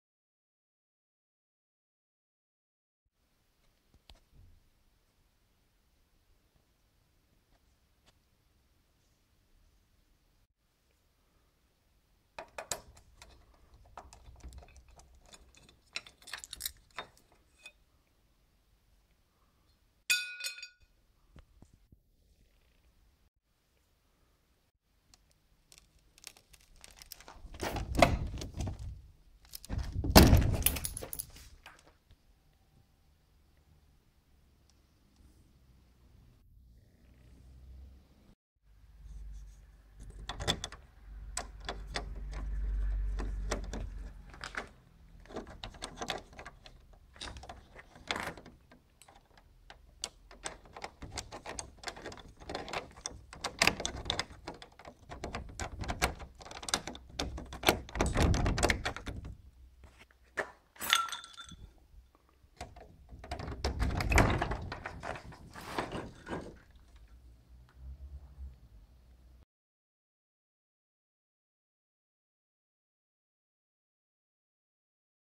Snapping euro cylinder with simple tools (Lomljenje cilindra brave)
Please don't think that this video helps thiefs because they alreday know this.

This is just my amauter try, and hope to spread awareness to replace your locks with more secure one's.
Lock in video Titan K
Tools needed are very simple and very easy to use.